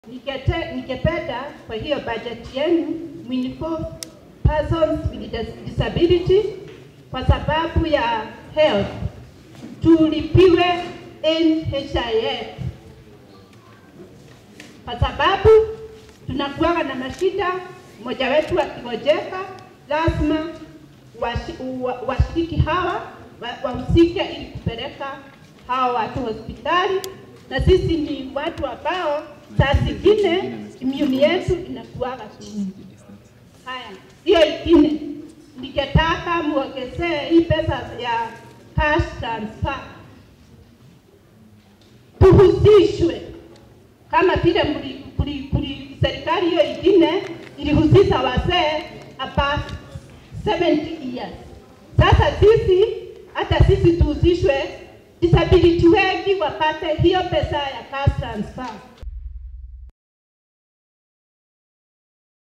Nyandarua residents table their views during public participation on the 2023/2024 Budget
The Parliamentary Budget Committee is keen on incorporating the views, and thoughts of the public in the 2023/24 Budget to assist in the improvement of service delivery to the people of Kenya, and in enacting policies that will keep the government accountable.